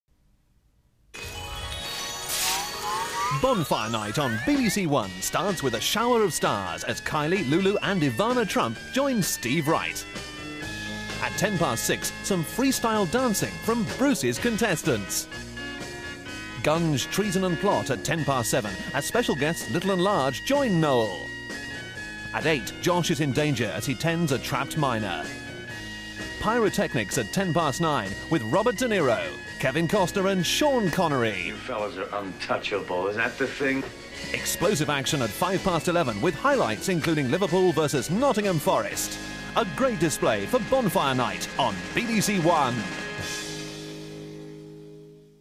Trailer: BBC1 Saturday Night 5th November 1994
This trailer was shown at 1:55am on the 5th November 1994 on BBC1.